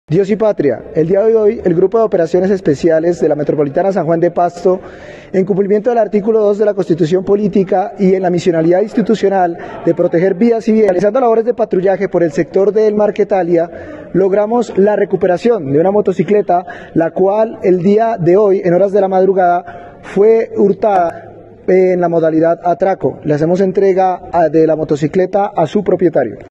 💯 RECUPERAN MOTO ROBADA EN PASTO
La Policía Nacional en Pasto, a través del Grupo de Operaciones Especiales GOES, mediante acciones de control, logra un resultado operativo de alto impacto, que permitió la recuperación de una motocicleta hurtada en el sector de Marquetalia.

En una acción eficaz, el personal policial desplegó un exhaustivo control y registro en toda la capital Nariñense, como parte de su compromiso continuo con la seguridad ciudadana.

La motocicleta, que había sido hurtada en las primeras horas de la madrugada mediante la modalidad de atraco, fue reportada oportunamente por su propietario a través de la línea de emergencia 123. La pronta respuesta y coordinación de las uniformados permitió ubicar el vehículo en el mencionado sector de la ciudad de San Juan de Pasto.

El trabajo conjunto fueron clave para el éxito de este resultado operativo que culminó con la entrega de la motocicleta recuperada a su legítimo dueño, quien expresó su gratitud por la rápida actuación de los policiales.